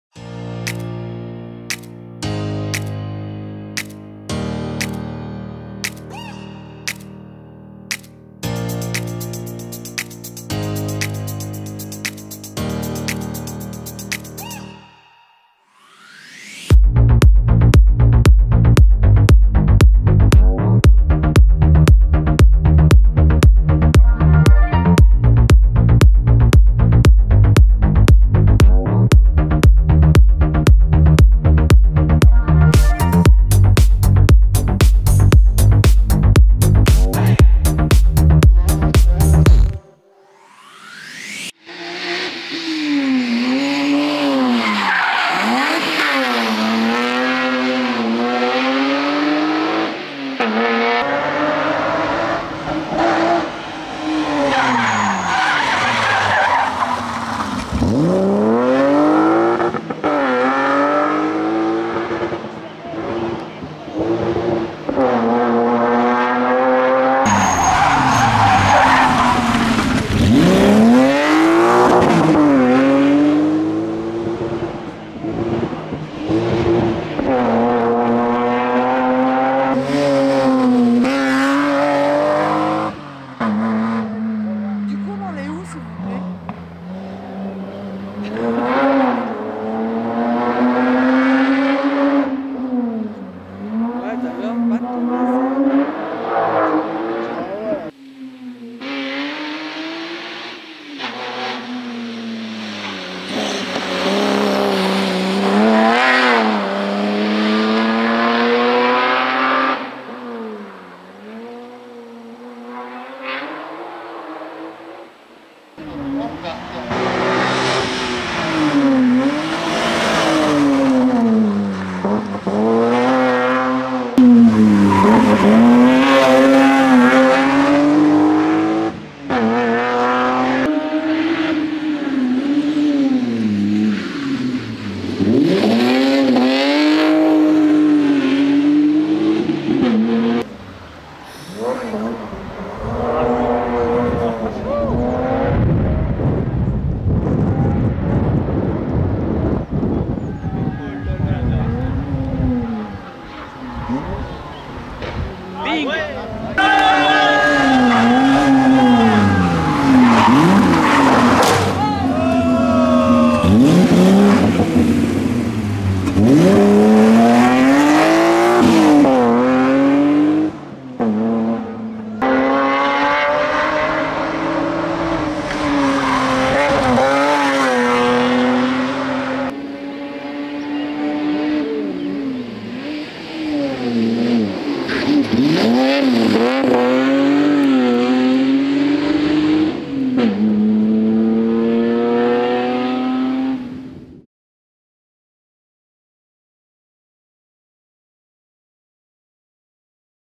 Rallye du Balcon Est - Est du Vercors 2022 - BMW 325I N°17 - Clément PAUTRIEUX et Maxence OLIVER
Voici la vidéo personnalisée avec les passages de Clément PAUTRIEUX et de Maxence OLIVER à bord de la BMW 325I N°17 lors du Rallye du Balcon Est - Est du Vercors 2022.